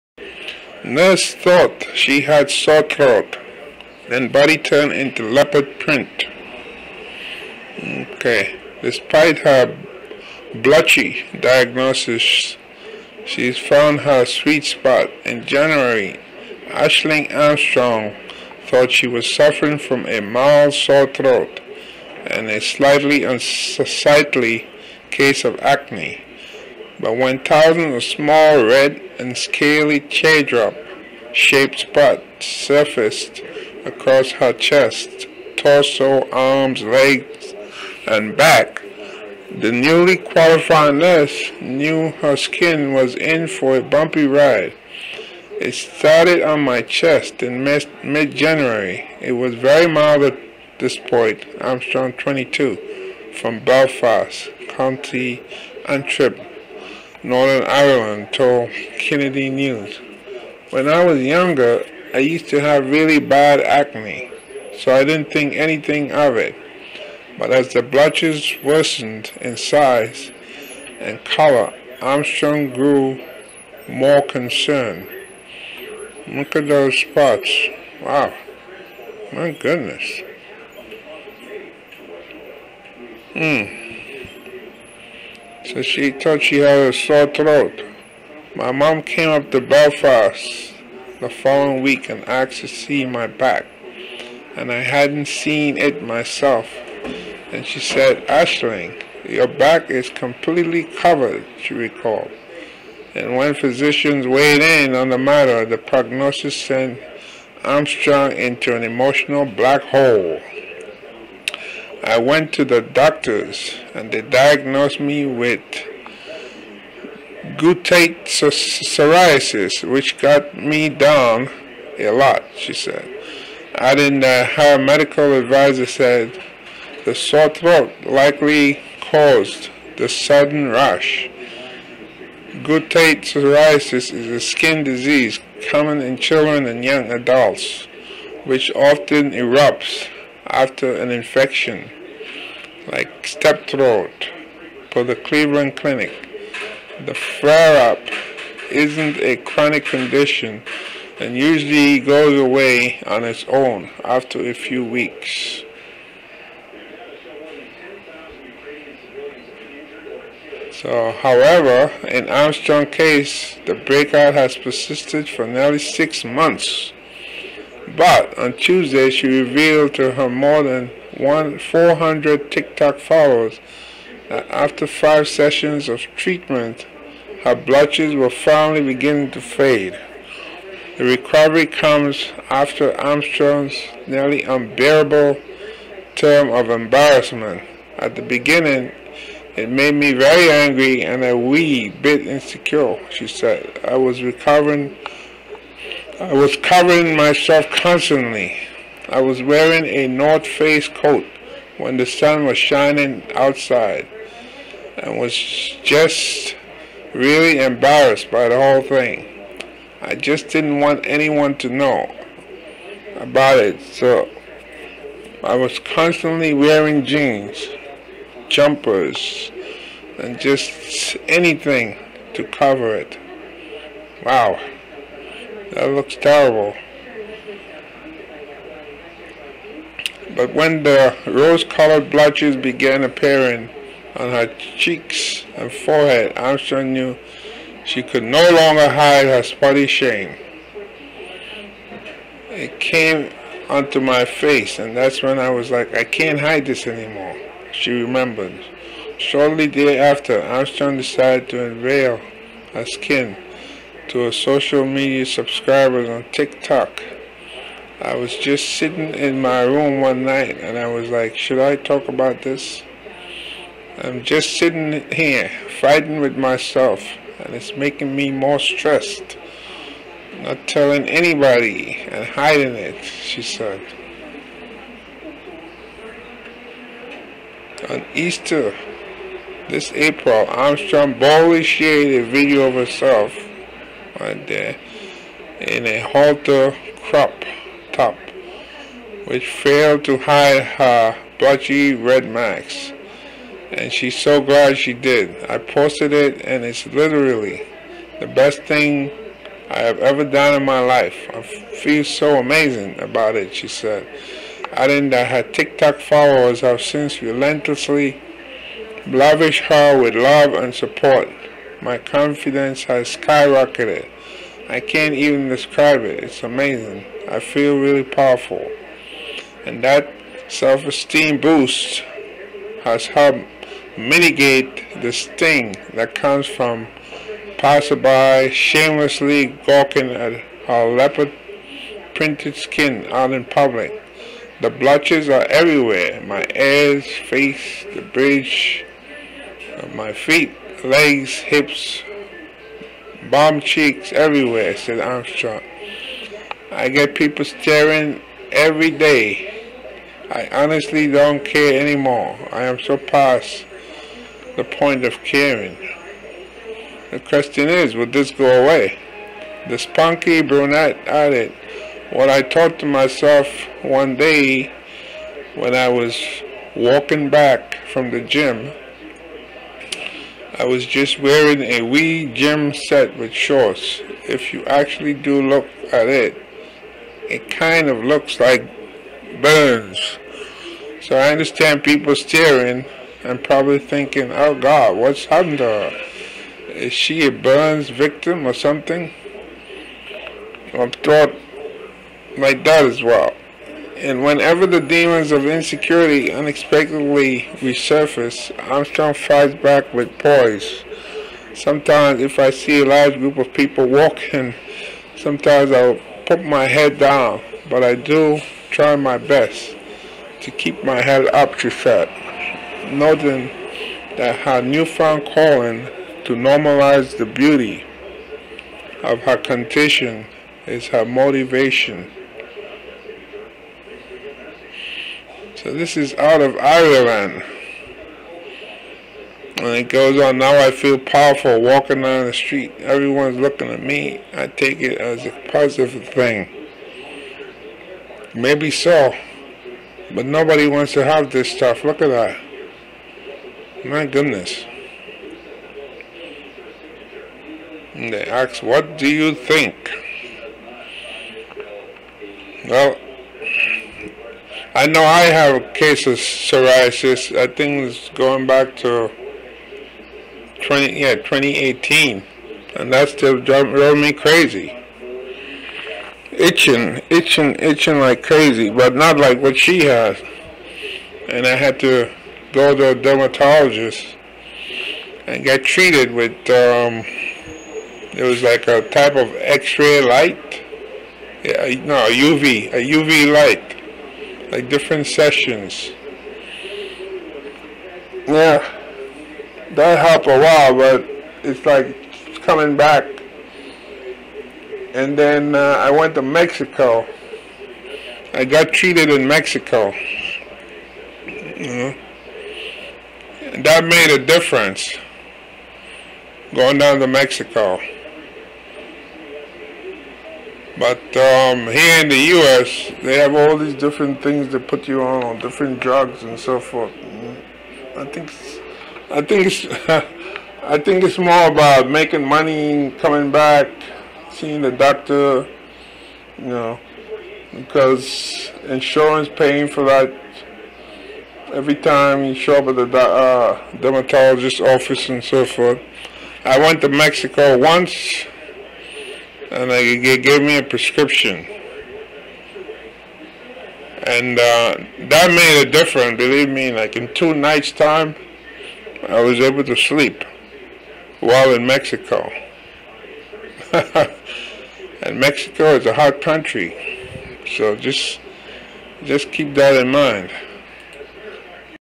Nurse With Leopard Skin
"“I went to the doctors and they diagnosed me with Guttate psoriasis, which got me down a lot,” she said, adding that her medical adviser said the sore throat likely caused the sudden rash.

Guttate psoriasis is a skin disease, common in children and young adults, which often erupts after an infection like strep throat, per the Cleveland Clinic. The flare-up isn’t a chronic condition and usually goes away on its own after a few weeks."